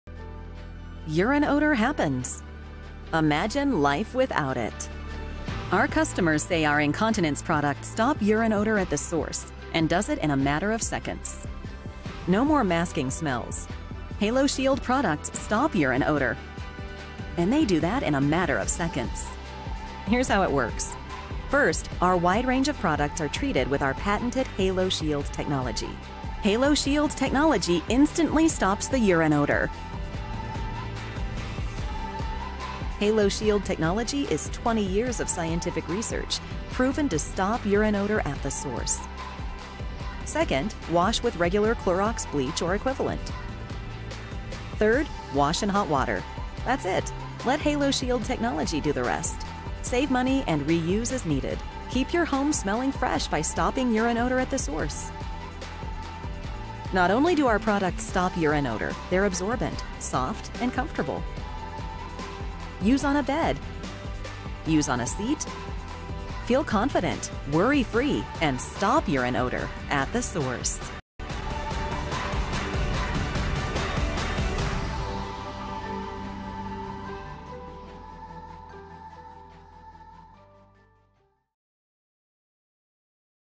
Haloshield Informational Video from AssistedLivingStore.com
Haloshield underpads are designed to stop unpleasant and embarrassing urine odors at the source. Developed by PhD scientists with over 20 years of research experience, the proven and patented urine odor killing power of these pads renews with each wash in regular chlorine bleach.